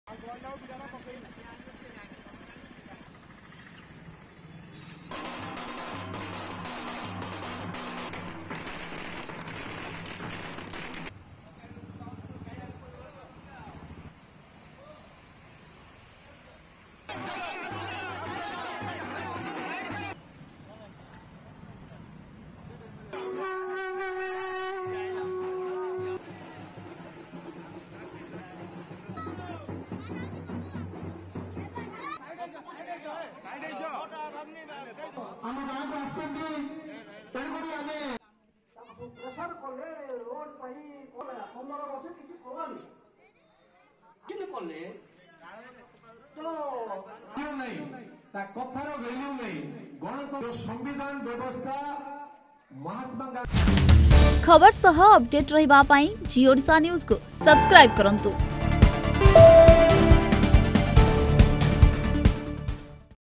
Berhampur MLA Attends Jan Samarthan Padyatra In His Constituency, Receives Grand Welcome
About Channel:-